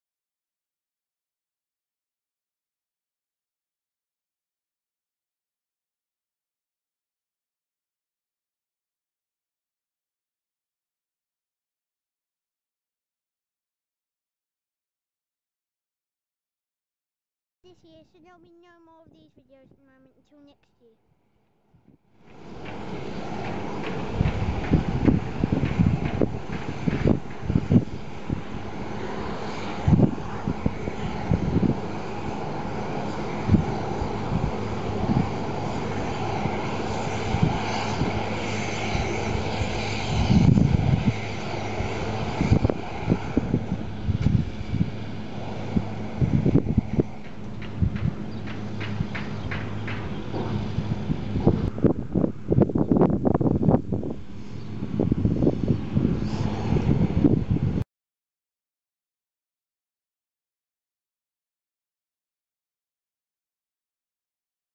Wrapping Bales for Haylage with Massey Ferguson 390T & Kverneland
Here's the other clip from 2012 and is (in my opinion) a bit better with quality than the baling clip and quite short. Massey Ferguson 390T & Kverneland UN7655 wrapping bales for haylage. Filmed in Cumbria, UK.